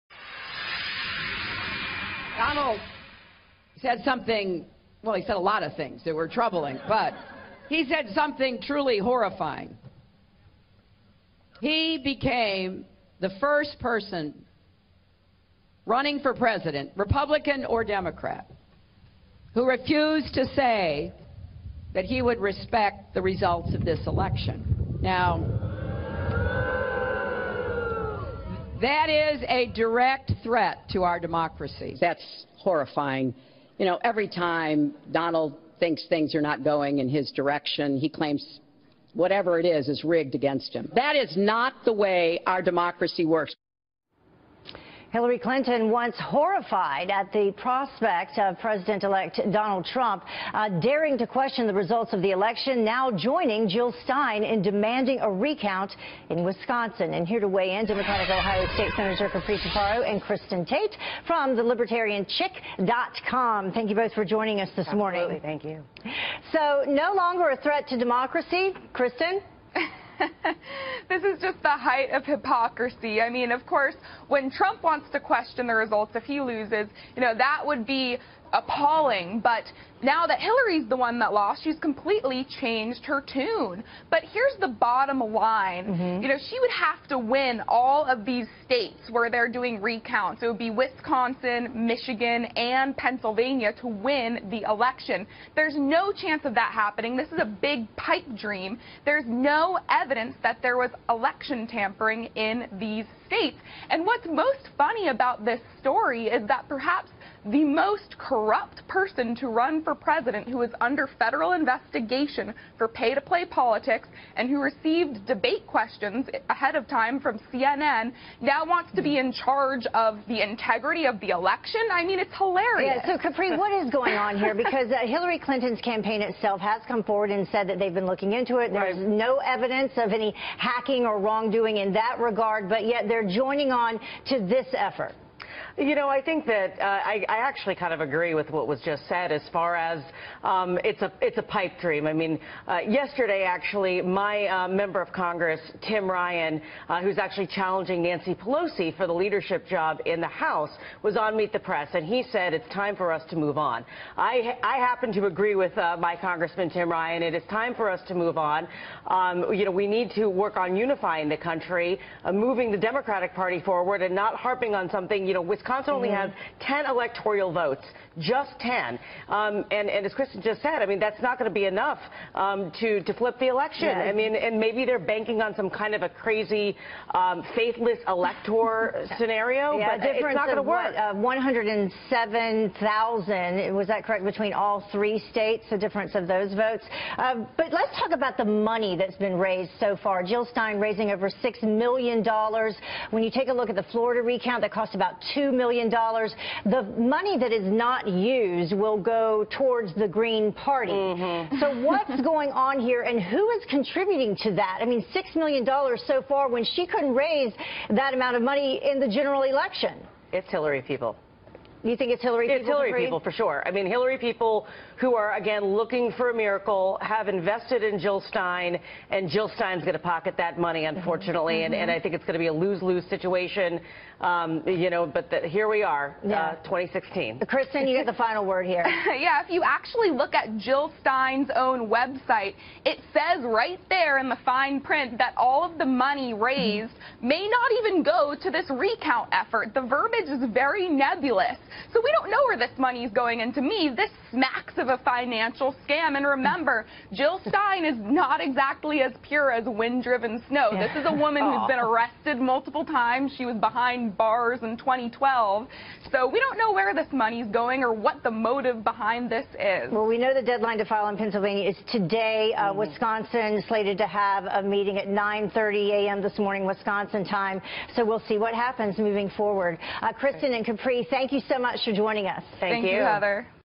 Hillary Clinton joins Jill Stein's recount efforts
▬ January 20, 2017; 54 days from now ▬